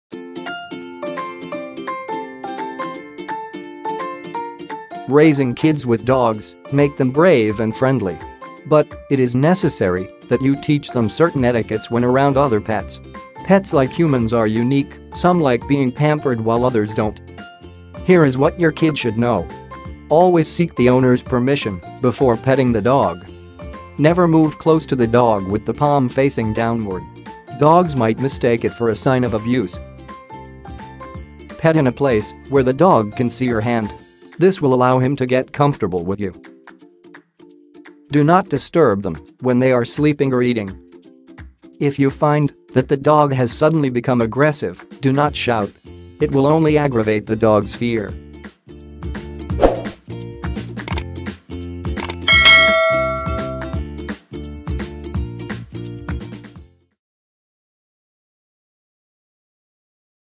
Dog safety tips to teach kids | Nimble Pet App
Kids and dogs make good friends. Even though Kids treat their furry friends as their playmates, we need to be cautious about certain things to ensure safety for both of them. Even a small misunderstanding can make dogs aggressive and make harm to your kids. Check out these tips to teach your kids how to befriend their furry companions safely.

Get Our app from

Let’s Connect